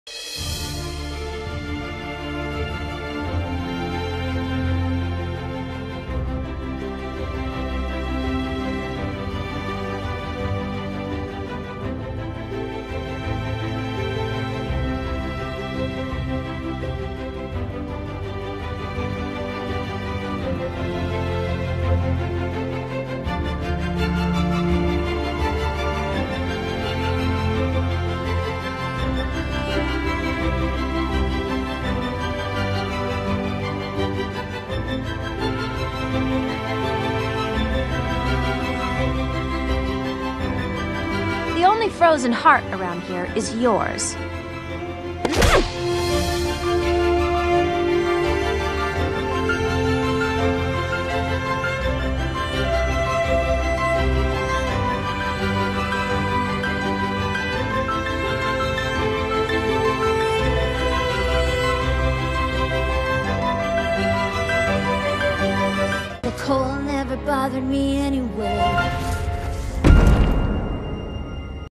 Elsa Was Originally Intended To Be The Villain In 'Frozen' | NowThis
Elsa Was Originally Intended To Be The Villain In 'Frozen' - "The problem was that we felt like we had seen it before."